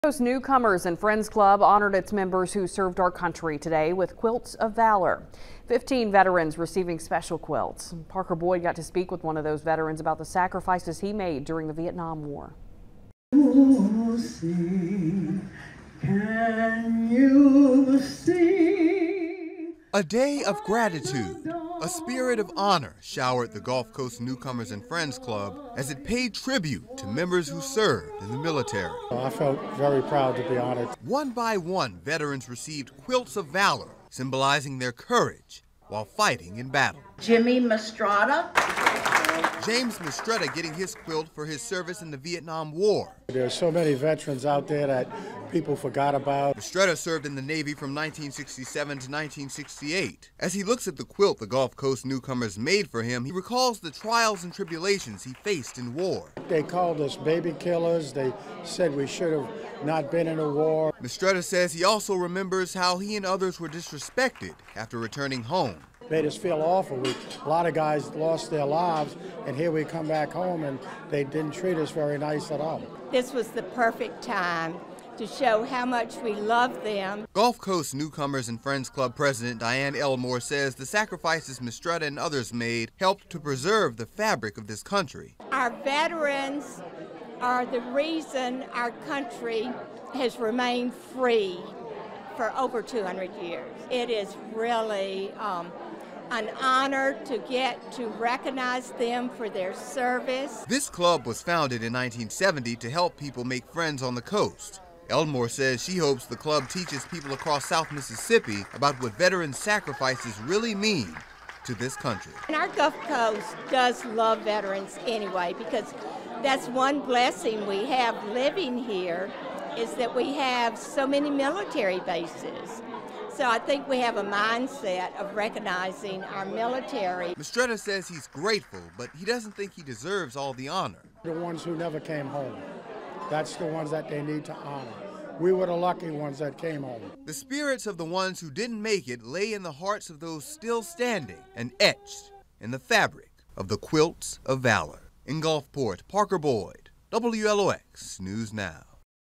Gulf Coast Newcomers and Friends Club honors members
The Gulf Coast Newcomers and Friends Club honored its members who served our country today with quilts of valor.